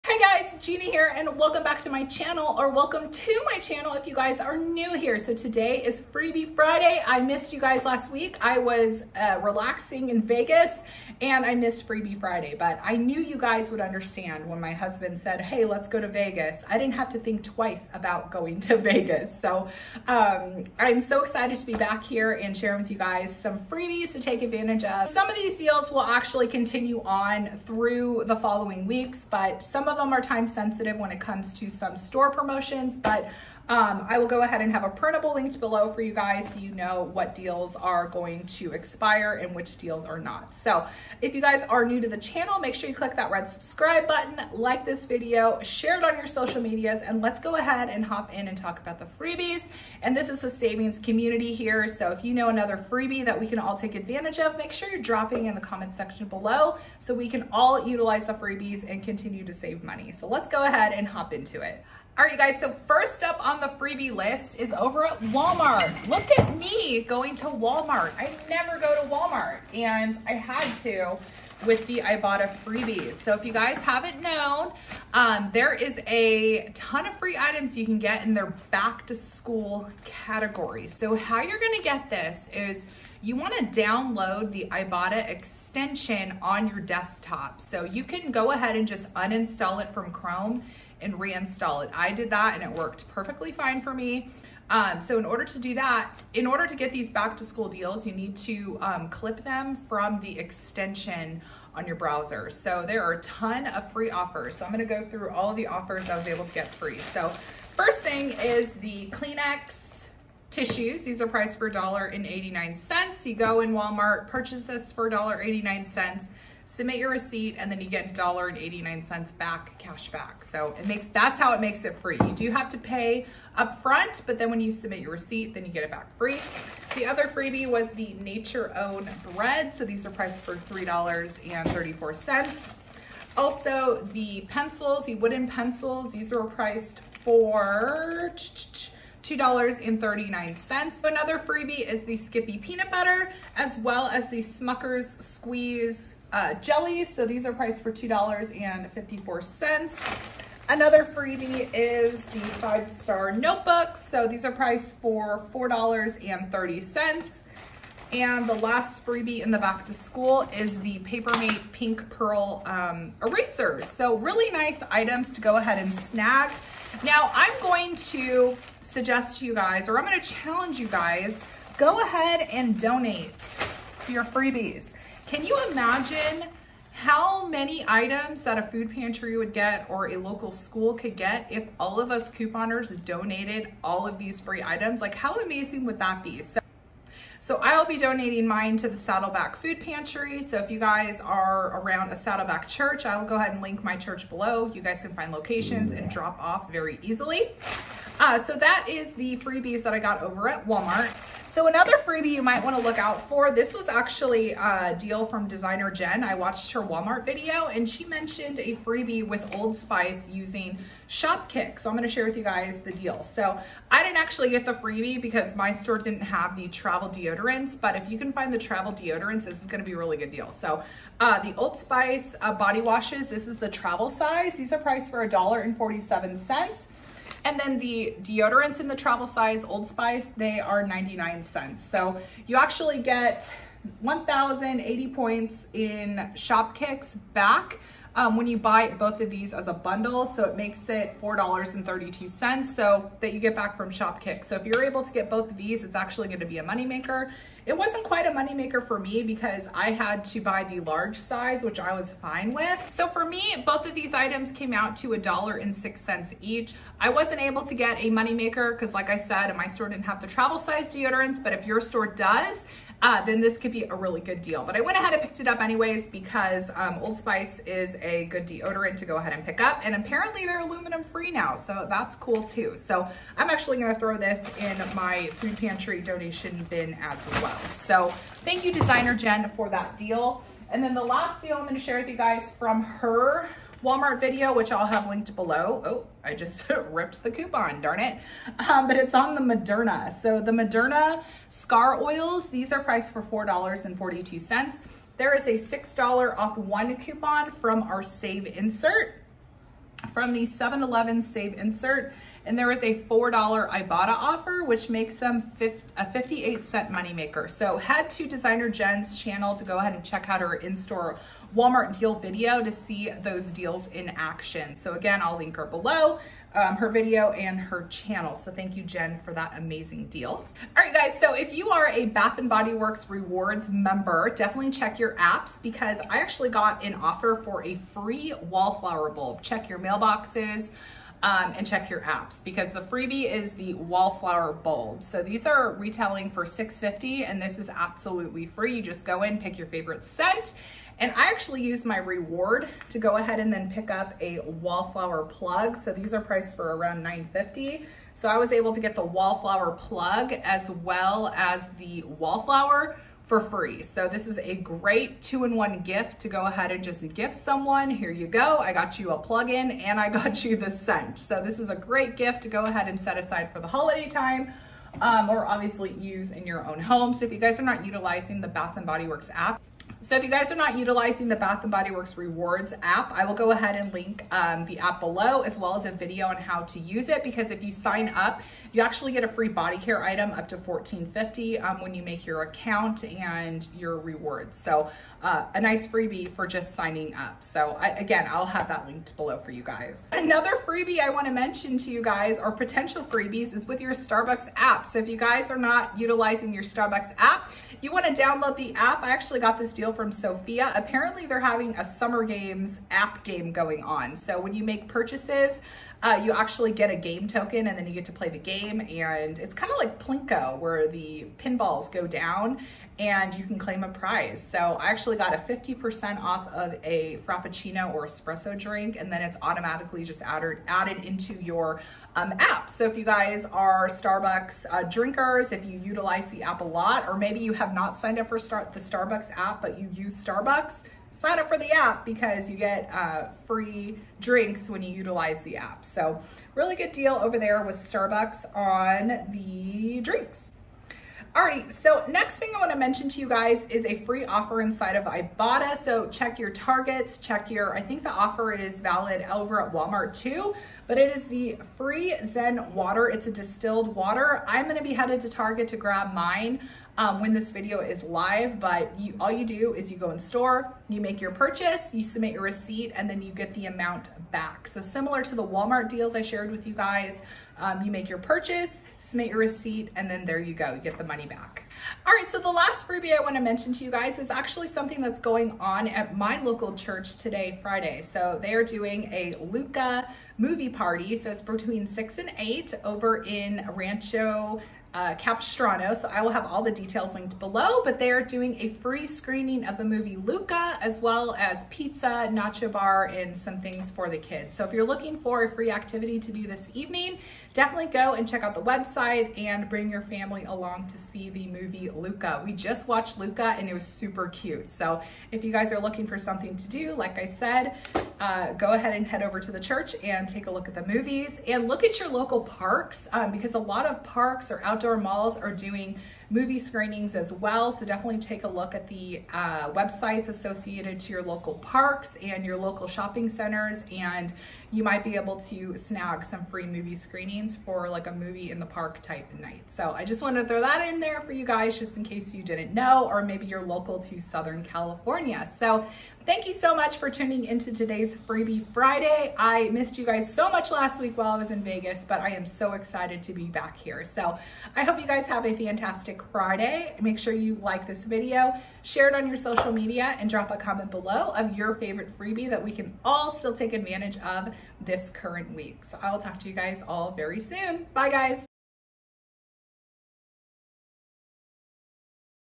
13 Freebies! ~ Freebie Friday ~ What's Free This Week?
13 Freebies!  ~ Freebie Friday  ~  What's Free This Week?

Rebate apps mentioned:

Fluz
Sign up for Fluz and make your first purchase to be entered to win $50 Nordstrom gift card

jqbzrb